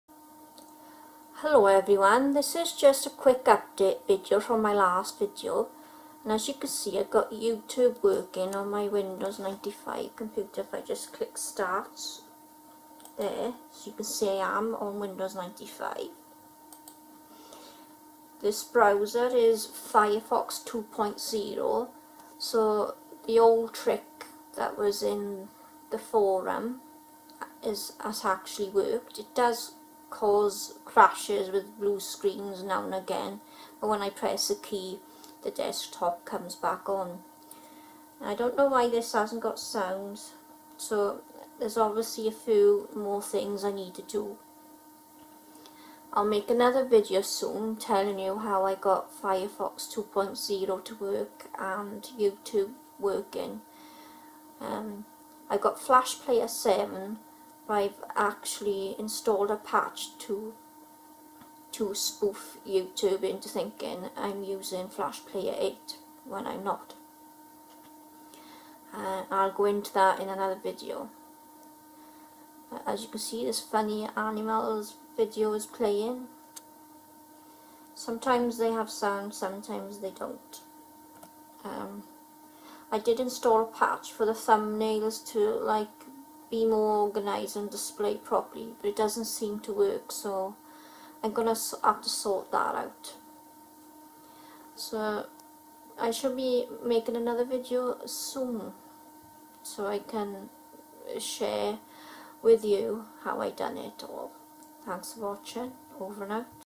Firefox 2 and Youtube on Windows 95 (Quick update video)
Firefox 2.0 is meant for windows 98,some windows 95 update files (some old links didn't work so I searched google for them) are needed,two files need to be deleted and two registry files edited etc for it to work. I also installed patches for Firefox to handle HTML5,spoof flash player patch and for thumbnails to float better,I'll explain more in another video when I have time.